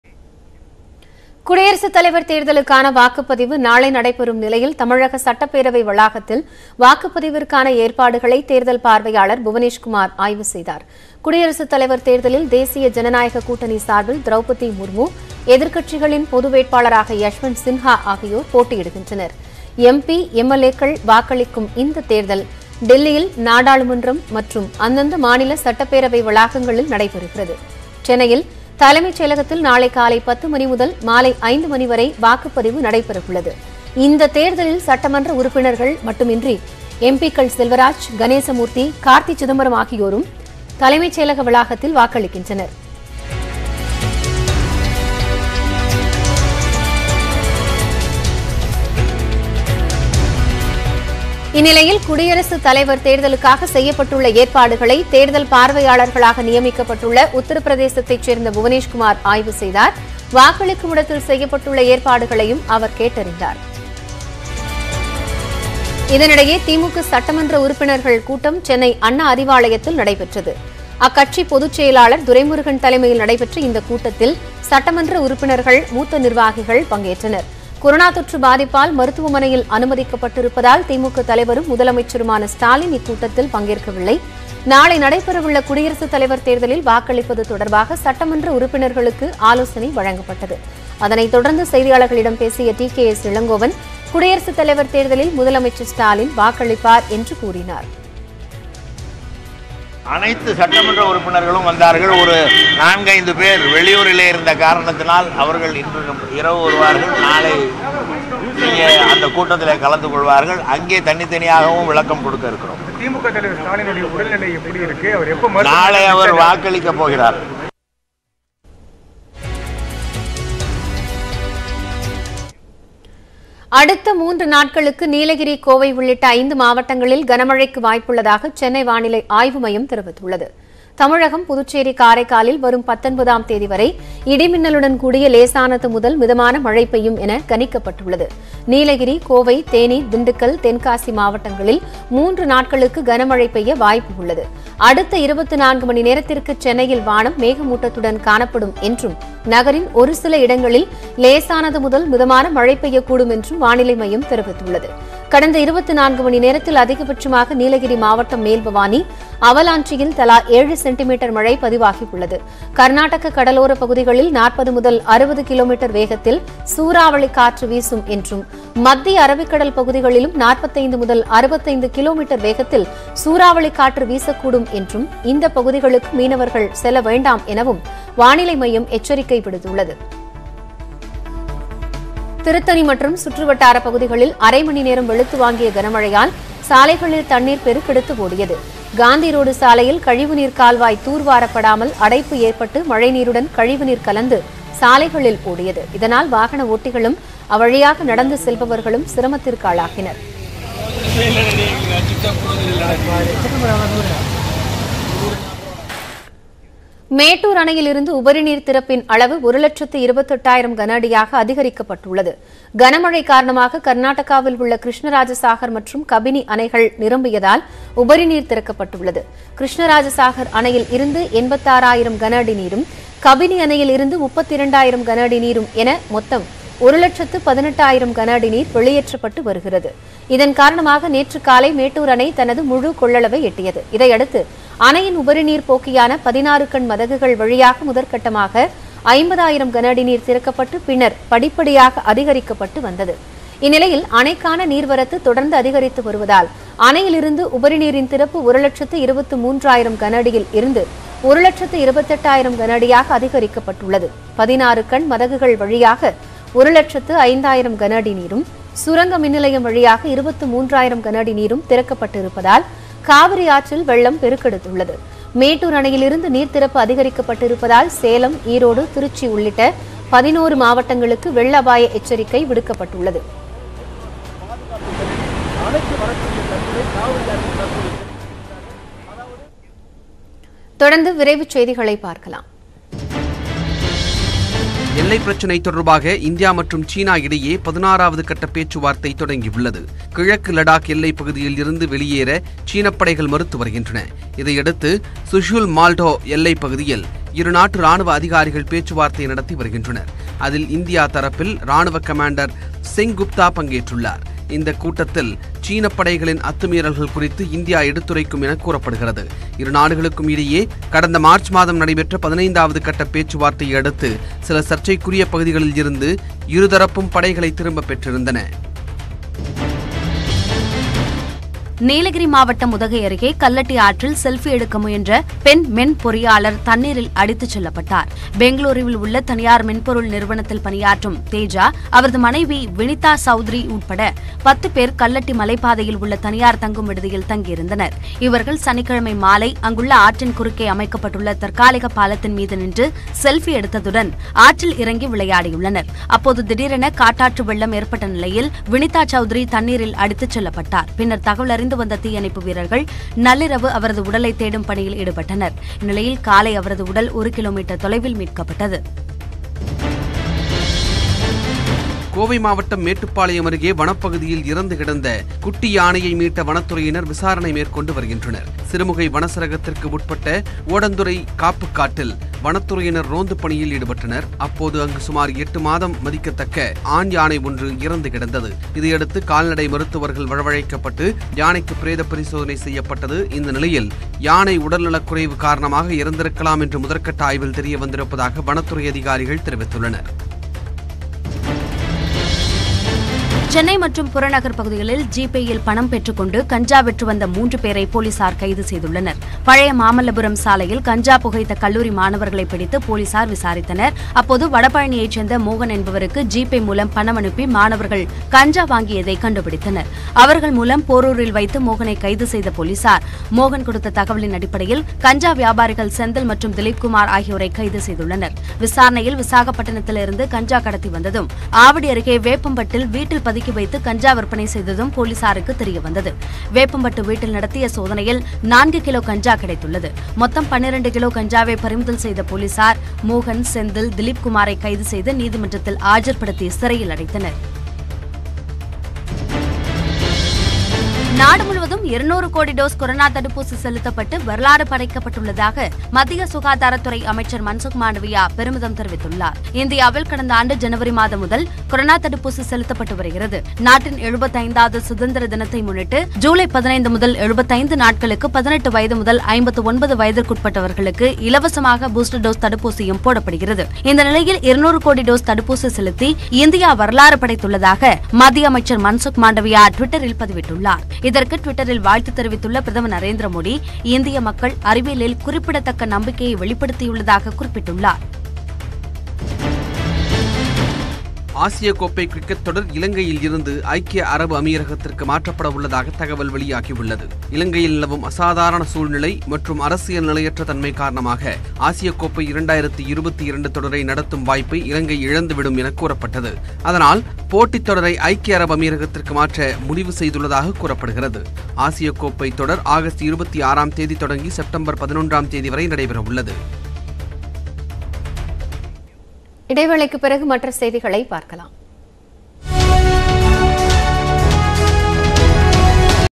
President Election 2022 | குடியரசுத் தலைவரை தேர்வு செய்ய நாளை வாக்குப்பதிவு
President Election 2022 | குடியரசுத்தலைவர் தேர்தலுக்கான வாக்குப்பதிவு நாளை நடைபெறும் நிலையில், தமிழக சட்டப்பேரவை வளாகத்தில் வாக்குப்பதிவிற்கான ஏற்பாடுகளைத் தேர்தல் பார்வையாளர் பார்வையிட்டார்